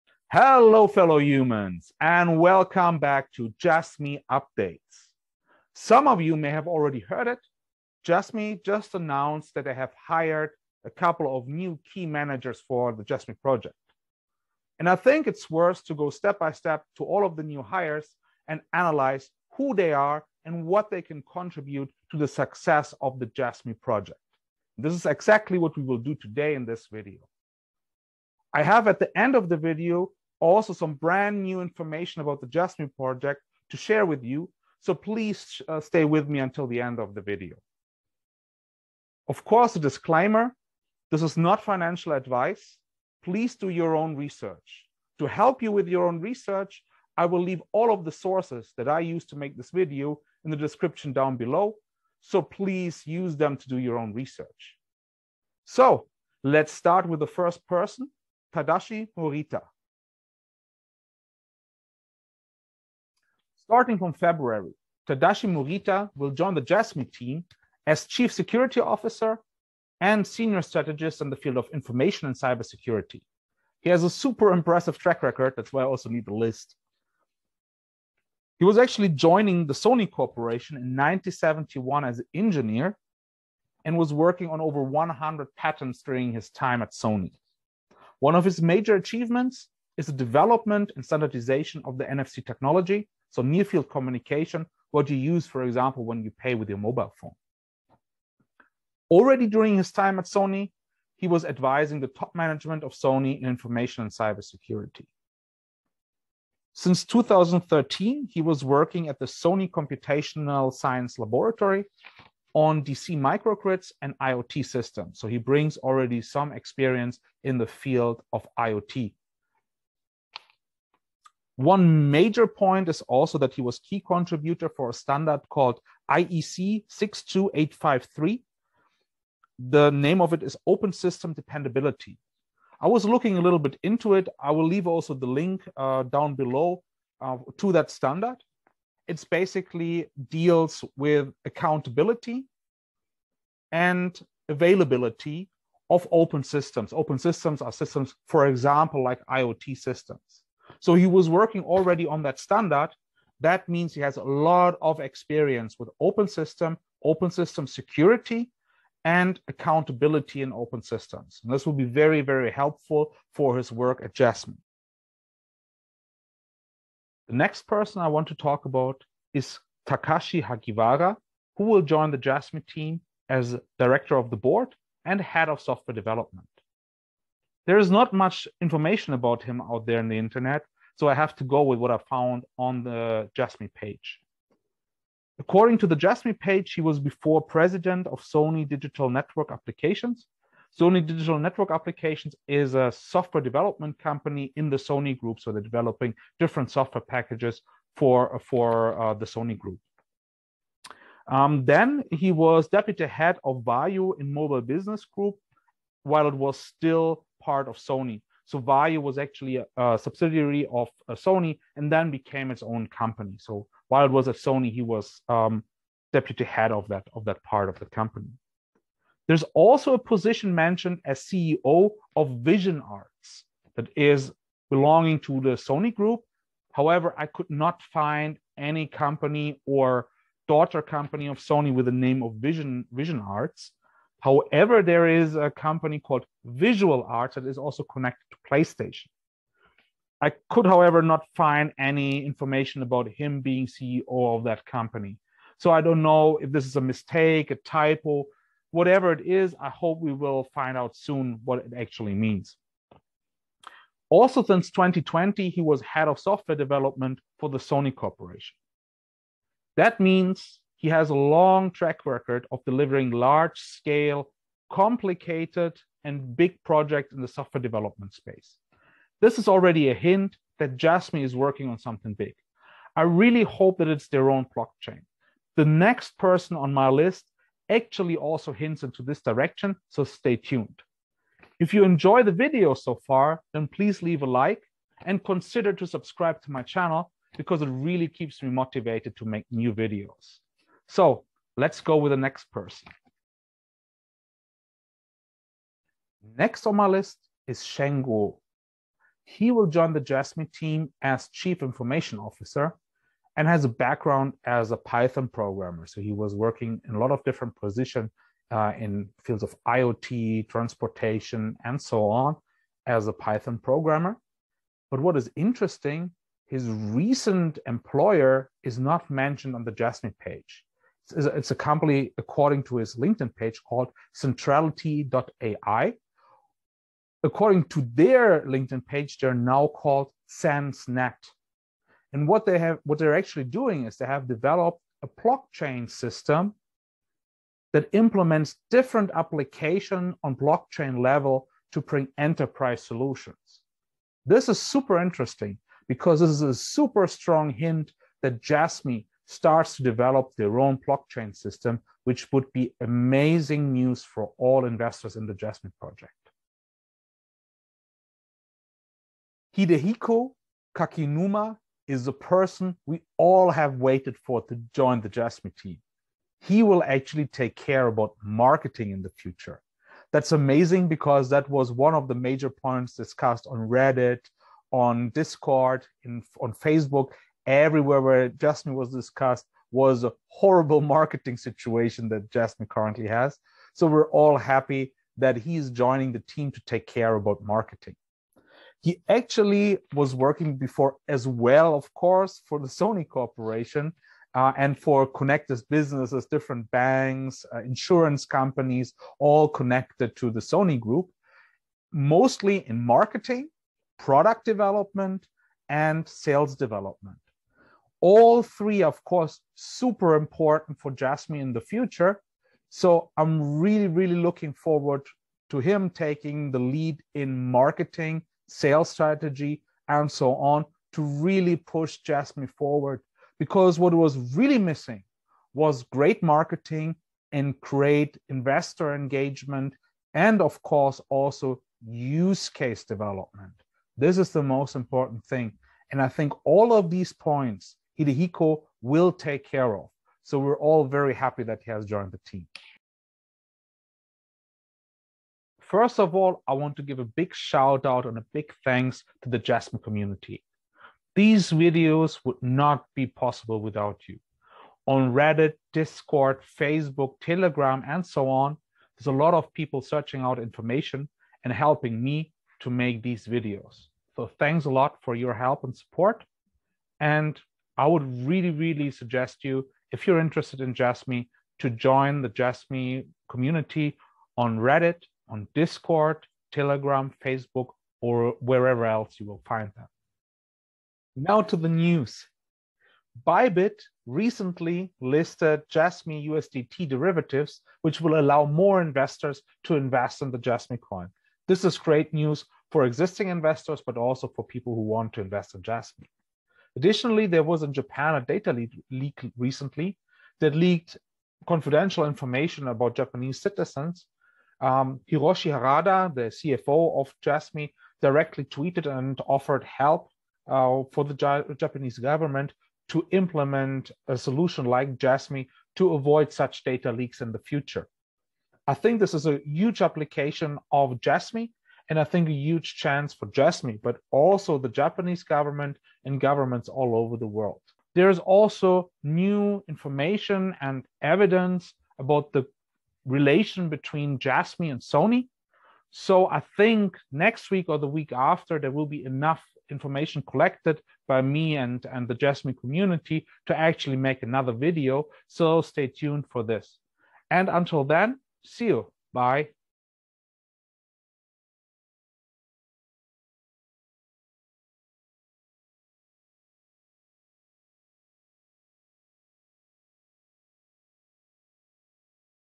Analyzing the new management team, Bybit and data leak - Jasmy Updates
Jasmy just hired new key managers, lets analyze them. Also, Bybit adds Jasmy derivatives and a data leak.